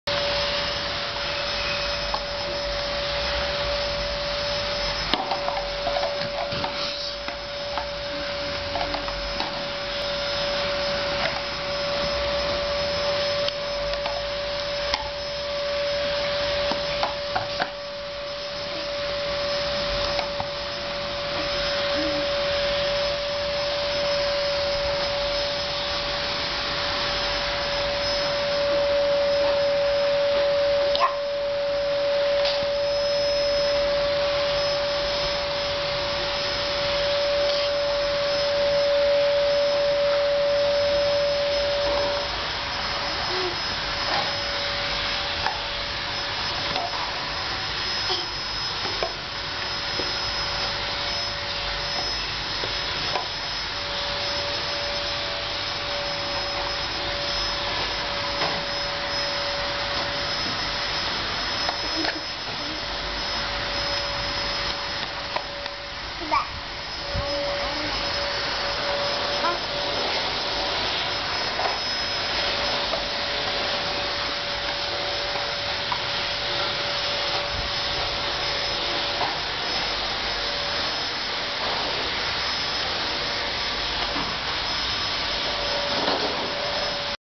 Kid with the blocks
She is playing with the blocks..